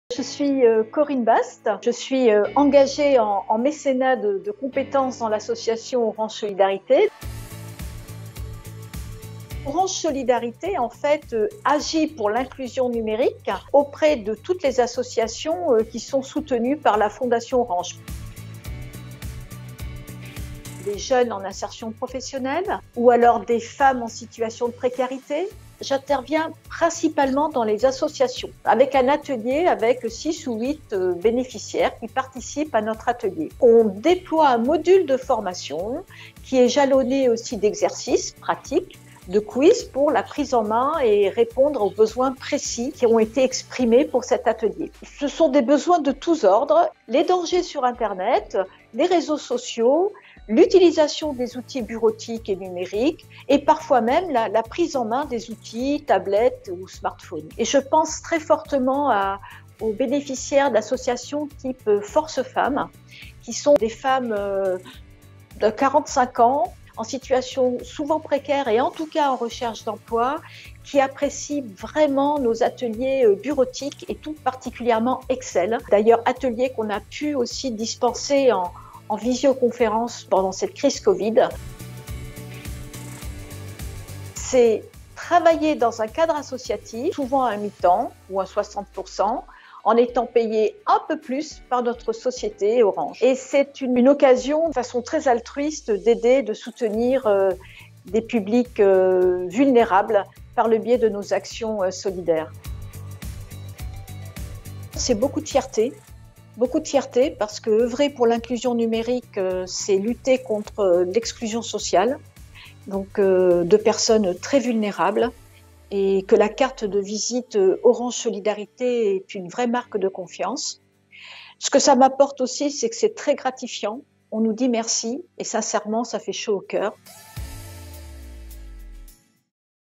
Corinne, salariée Orange en mécénat de compétences, formatrice en ateliers numériques…
serie leMessageDe : bénéficiaires, salariés engagés, associations partenaires… ils témoignent en vidéo.